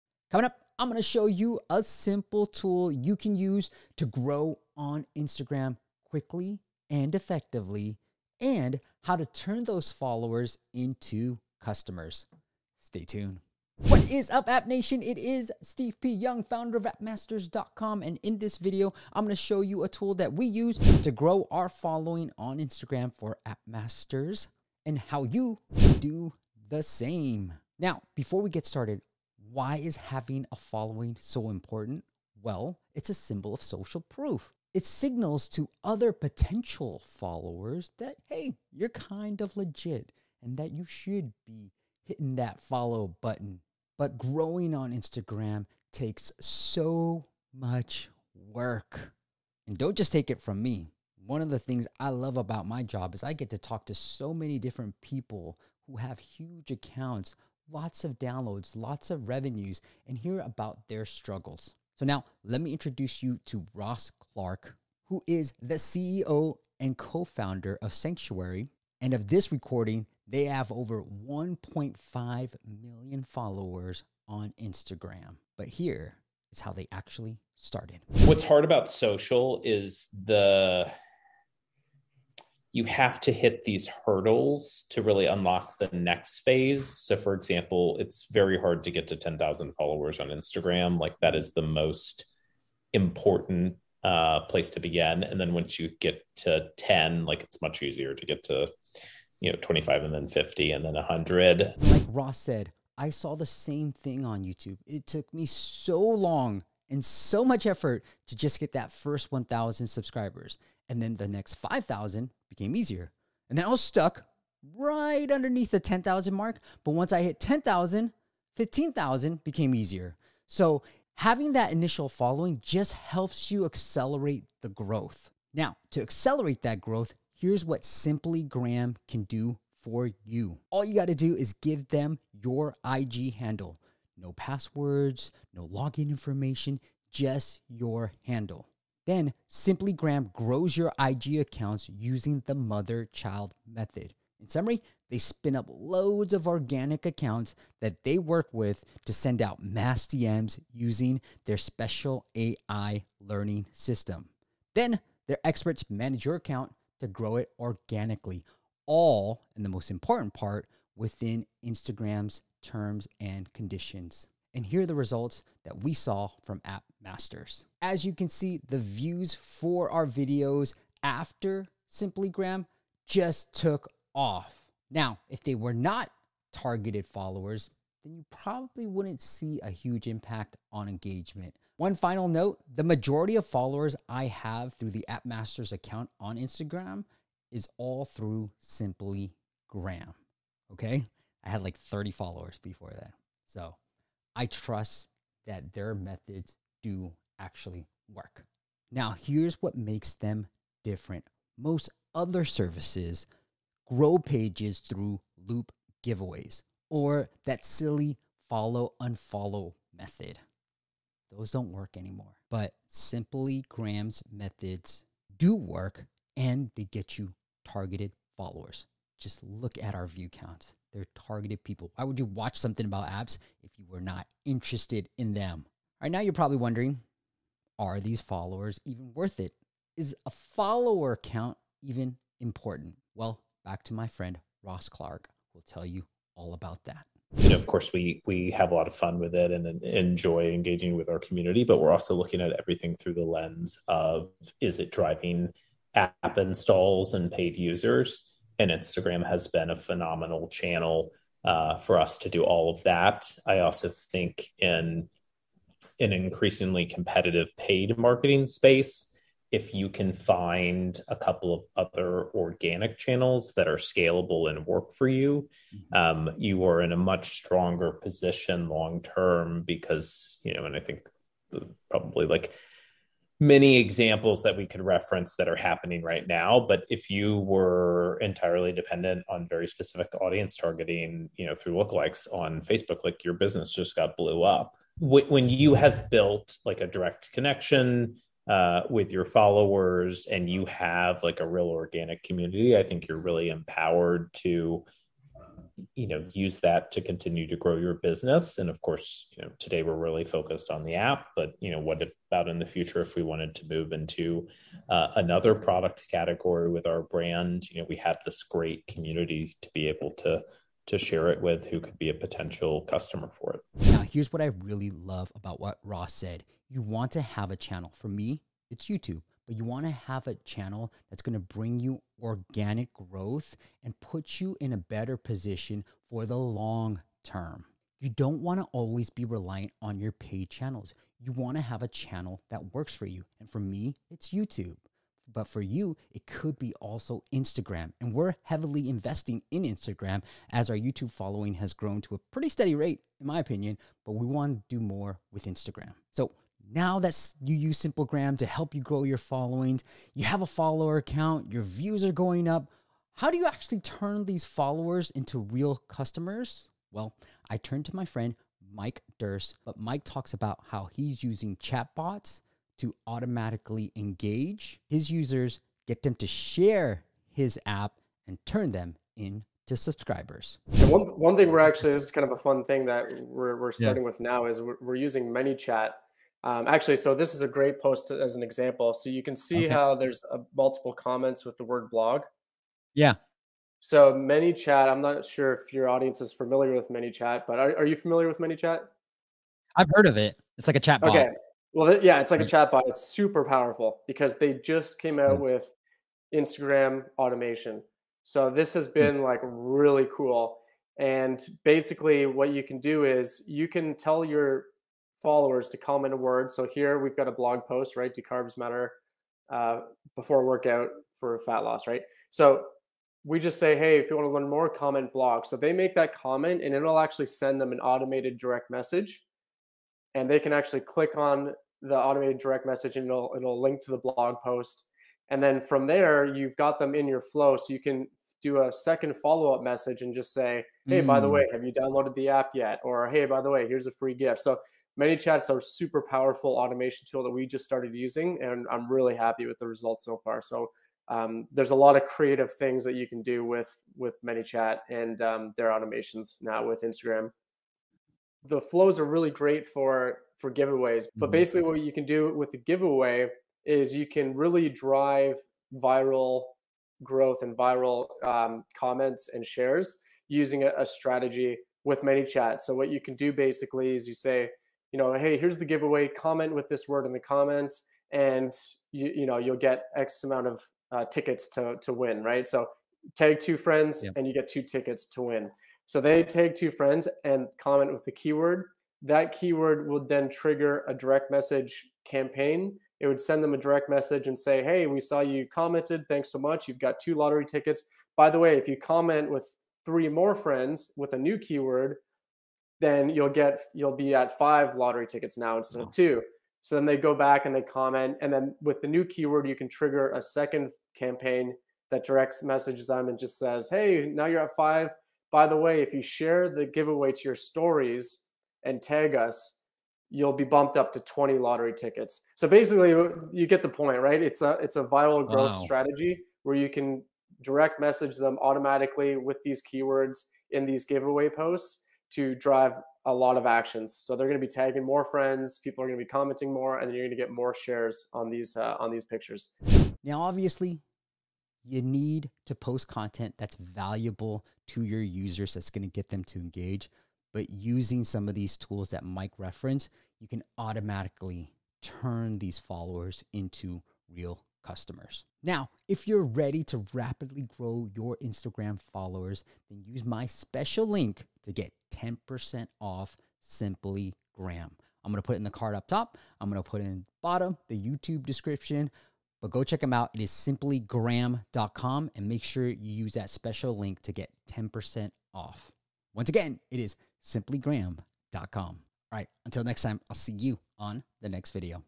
How to Grow on Instagram 2022 - The Fast & Organic Method
Discover how to grow on Instagram in 2022 - the fast and organic way!

You will also discover why it's important to grow on Instagram and how to automatically turn these followers into customers.

Simplygram grows Instagram accounts using the mother-child method. In summary, they spin up lots of organic accounts that they work with to send out mass DMs using their special AI learning system, their experts then manage your account to grow it organically, all within Instagram's T&C’s, promoting your IG page in order to generate followers, likes and comments.